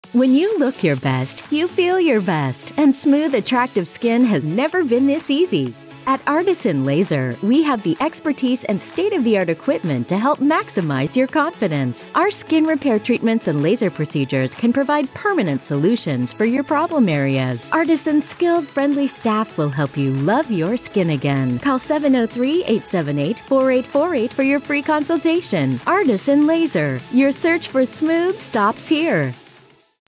Artisan Laser - Laser Hair Removal and Skin Renewing Treatments
Artisan Laser Skincare Center Medical Skin Care Laser Hair Removal Cable TV Commercial.